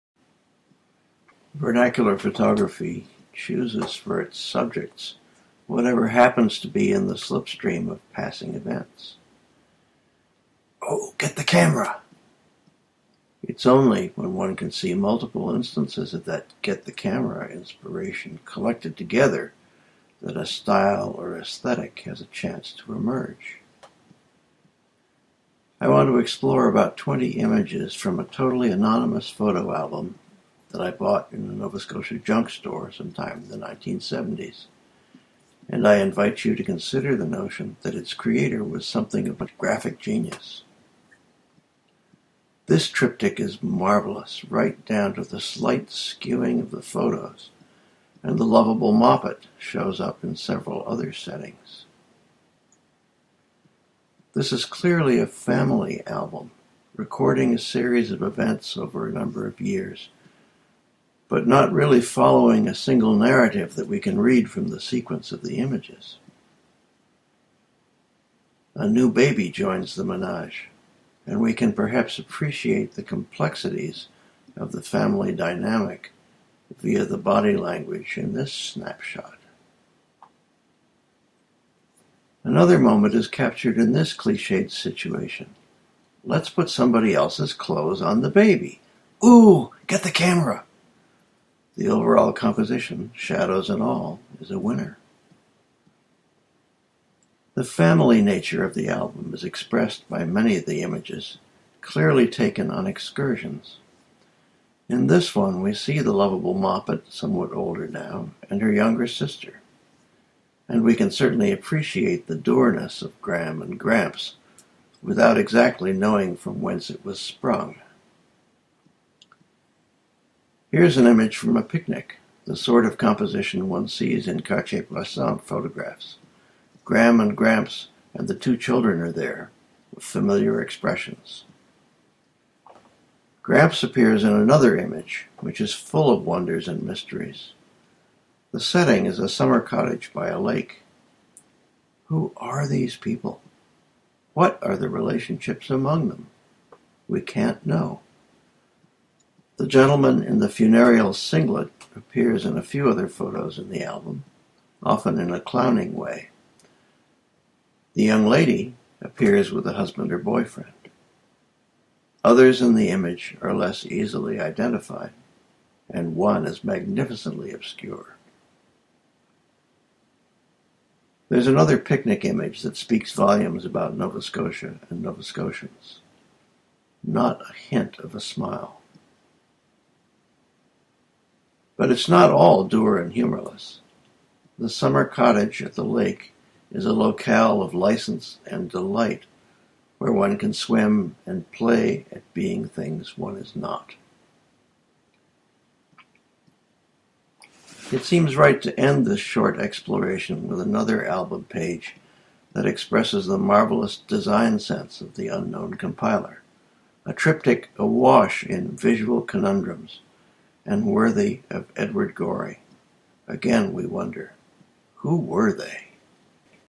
An Anonymous Photo Album
Found in a Nova Scotia junk store in the 1970s, teeming with wonderful images